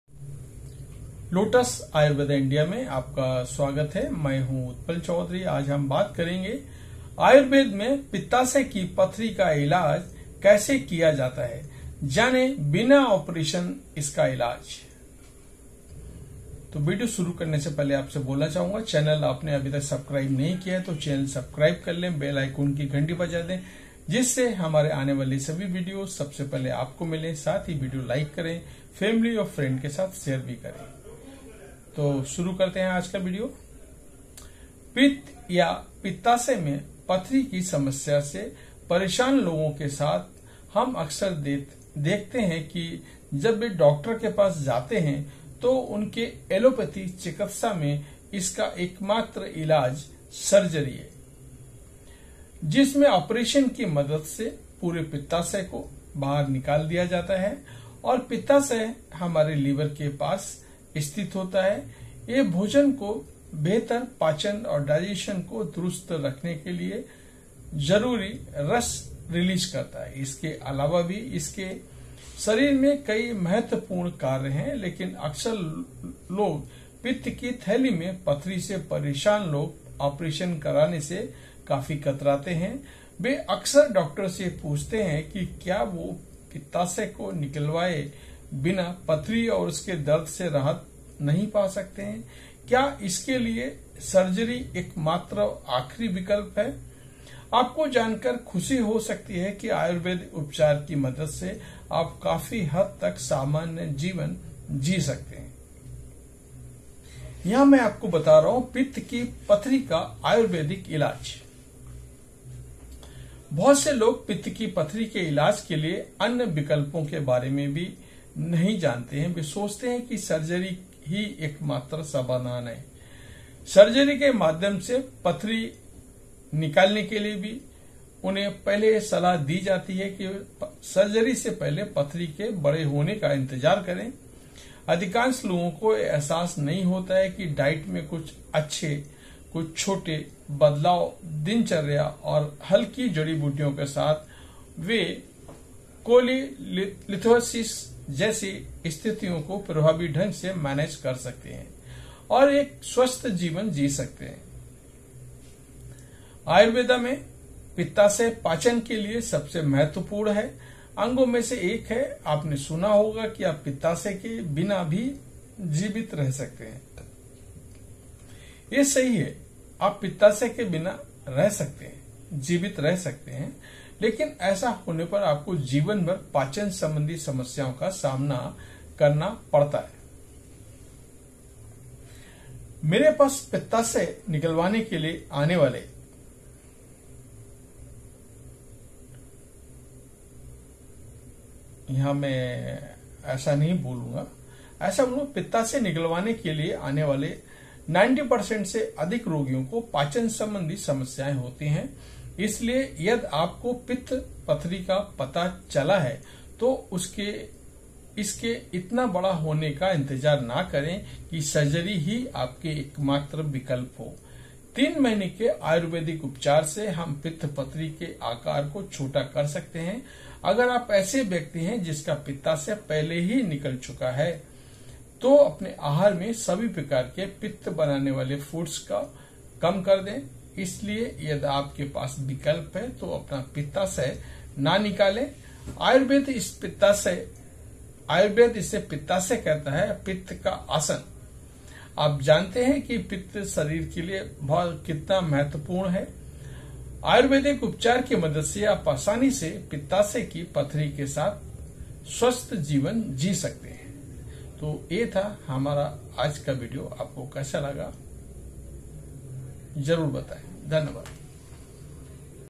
Gallbladder Stoneआयुर्वेद में पित्ताशय की पथरी का इलाज कैसे किया जाता है?जानें बिना ऑपरेशन इसका इलाज
Ayurveda Treatment For Gallbladder Stone | आयुर्वेद में पित्ताशय की पथरी का इलाज कैसे किया जाता है? जानें बिना ऑपरेशन इसका इलाज @LotusAyurveda_India